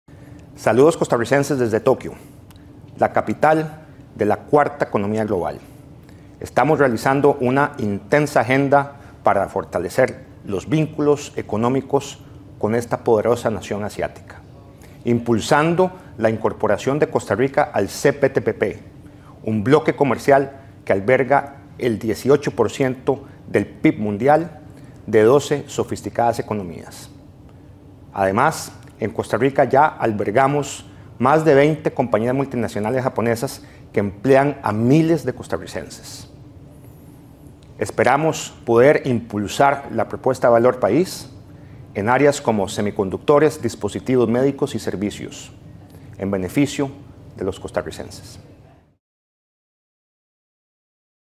Ministro Manuel Tovar
El Ministro Manuel Tovar junto con la Promotora del Comercio Exterior de Costa Rica aprovechan su gira por 🇯🇵 para visitar las casas matrices de distintas empresas japonesas instaladas en el país como Hitachi, Panasonic y Terumo.